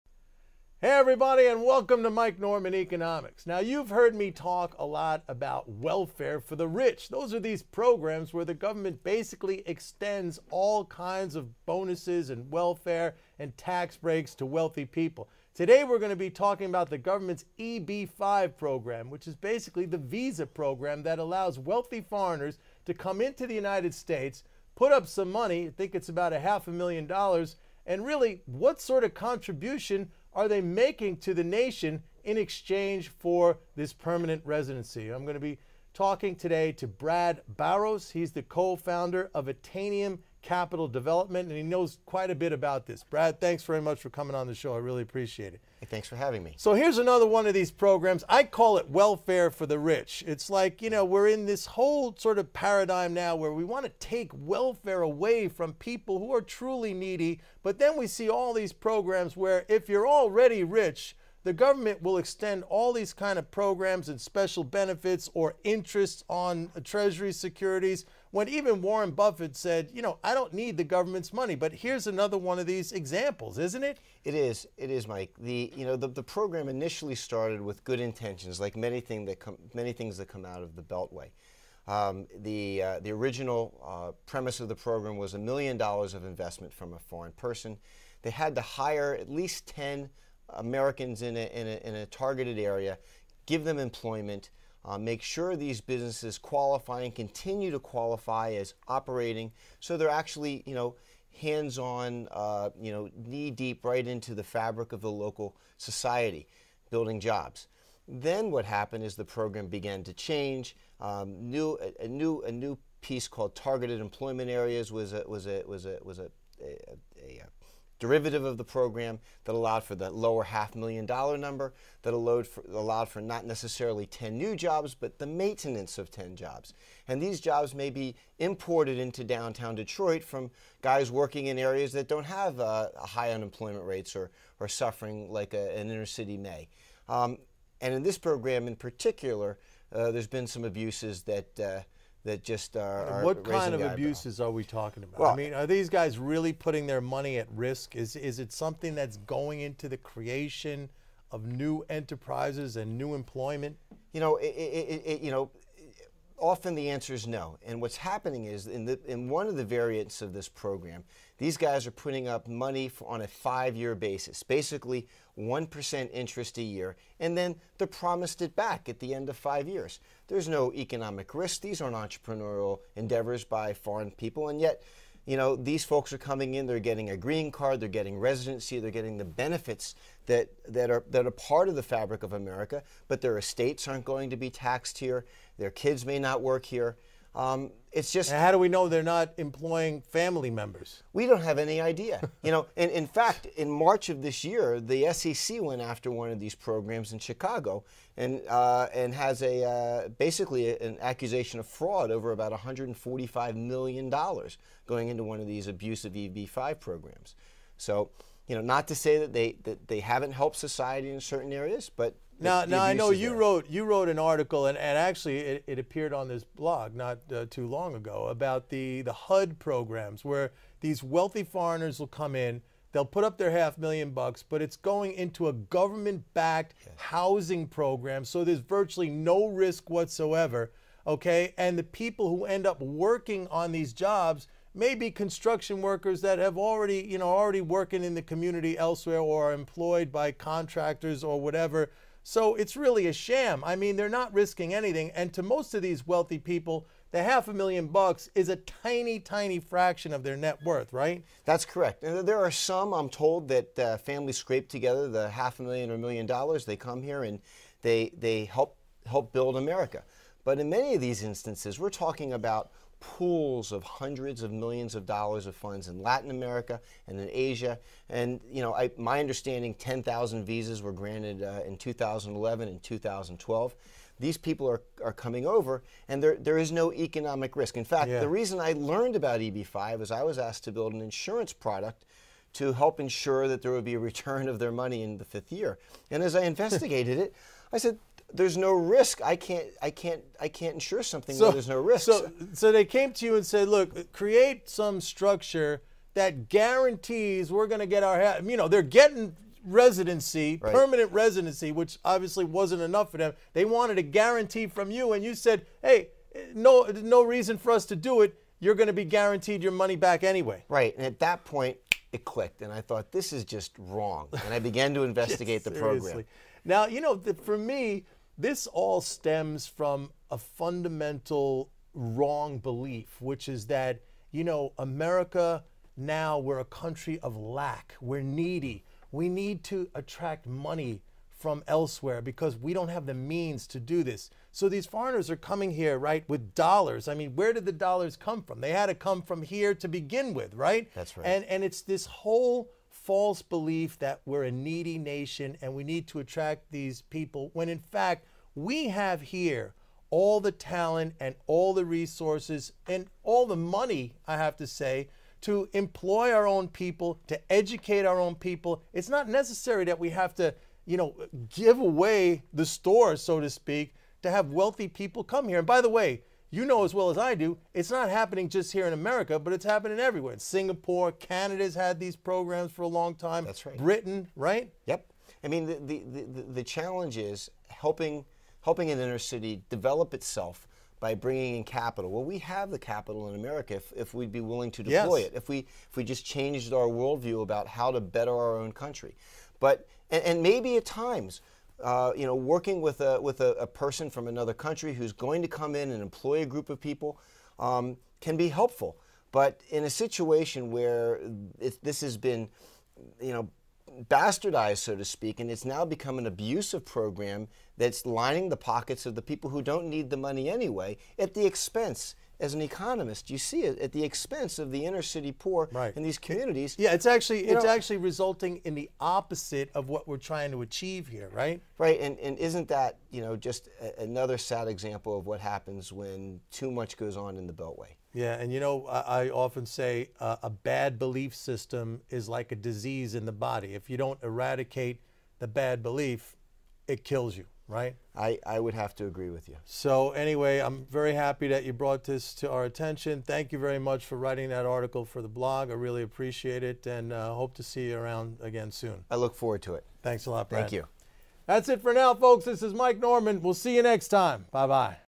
Mike Norman discusses U.S. visa program for the wealthy with Brad Barros
America's EB-5 program essentially allows wealthy foreigners to buy residency with little risk. Is this just another example of "welfare for the rich." Mike Norman discusses this with expert Brad Barros of Attainium Capital Development.